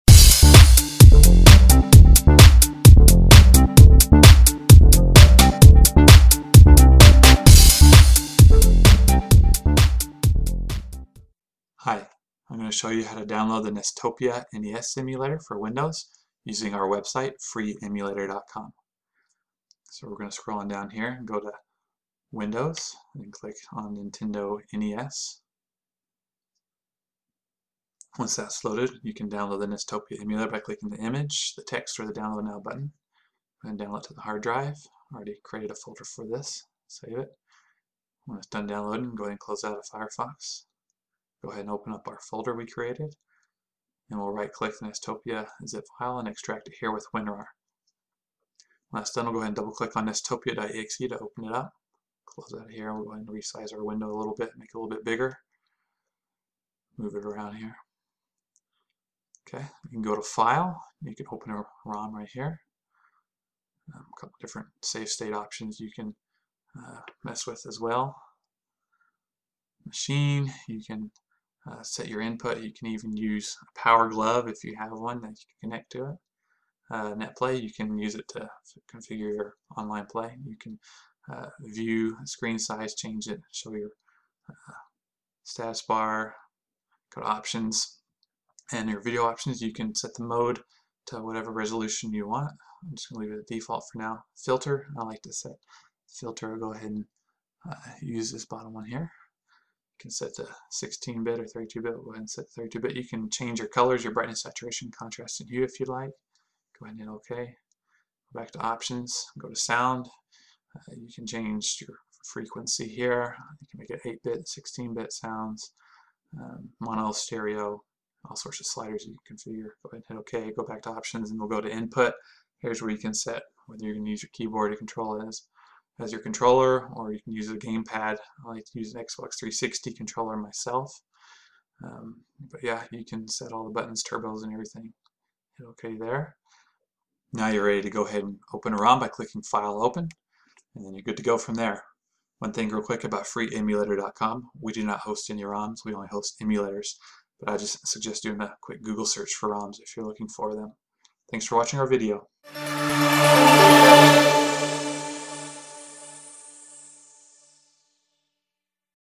Download and Configure Nestopia Emulator 1.40 for Nintendo NES on Windows [1080p HD]
Download Nintendo NES Nestopia 1.40 Emulator
In this installation tutorial video we show you how to download and quickly configure the Nestopia Nintendo Entertainment System (NES) emulator for Windows (version 1.40).  With Nestopia you can play all your old school favorite games, such as Super Mario Bros. 3, The Legend of Zelda, Super Mario Bros., Mega Man 2, Castlevania III: Dracula's Curse, Metroid, Mike Tyson's Punch-Out!!, Dragon Warrior, Bionic Commando, Contra, Kirby's Adventure, & DuckTales.

If you need WinRAR to extract any files from the video, you can get it from here: